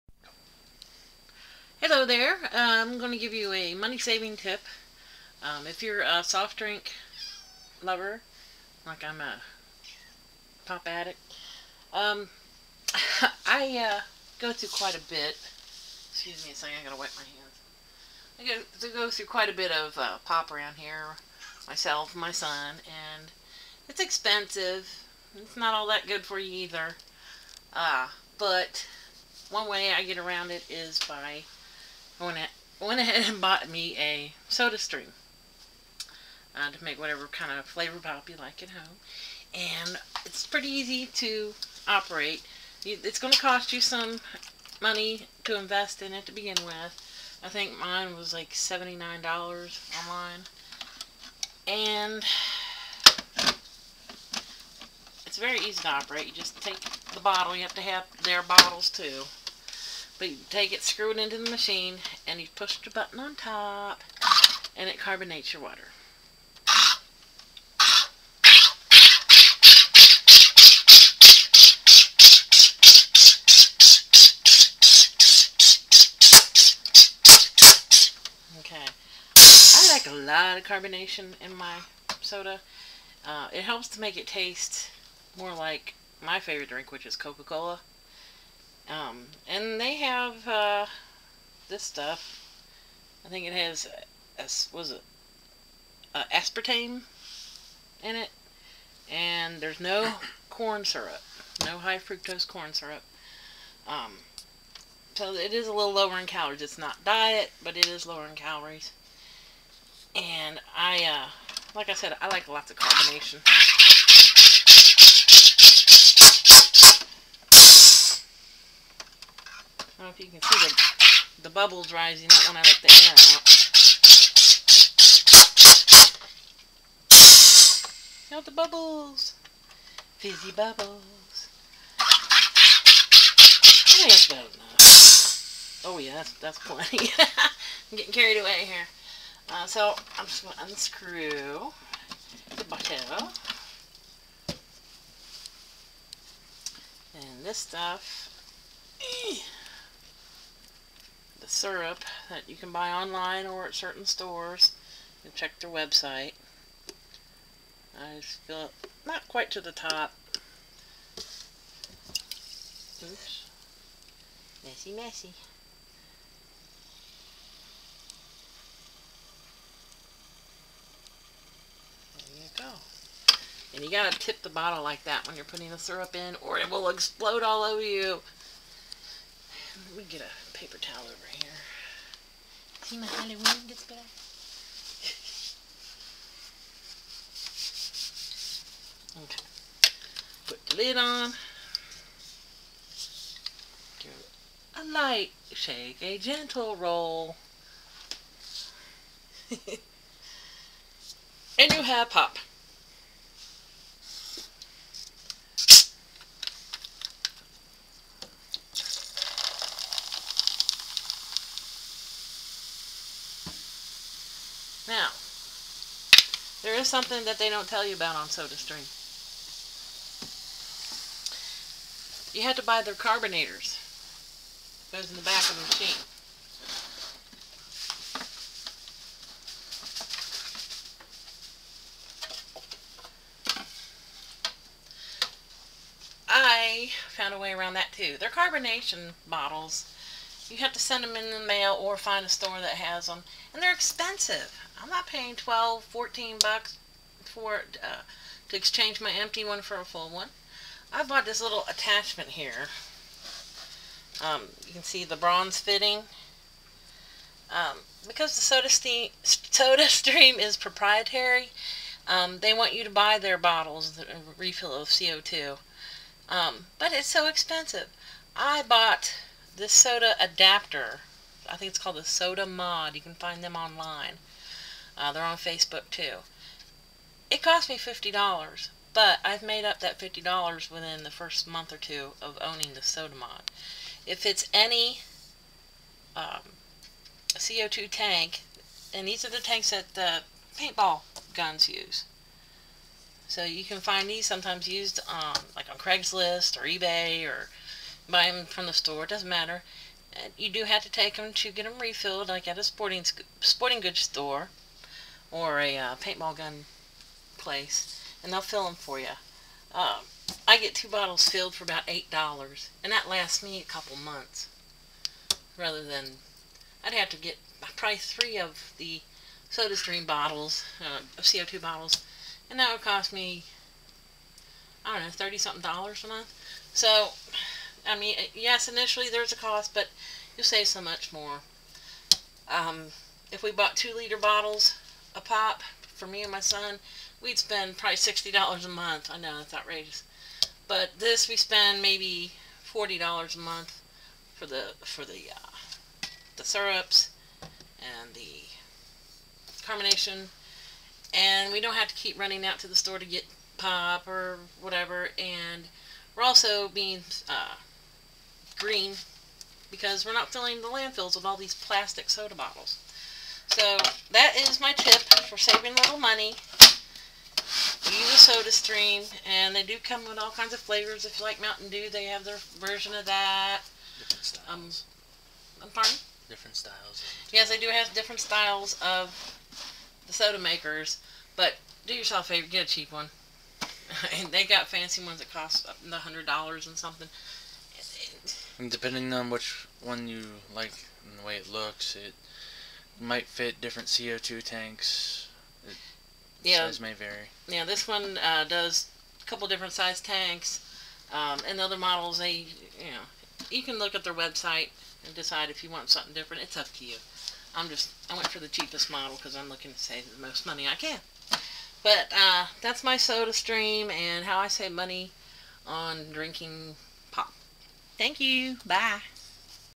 Make Your Own Soft Drinks At Home
See how using a Soda Stream with a Soda Mod will save you money if your household drinks a lot of pop.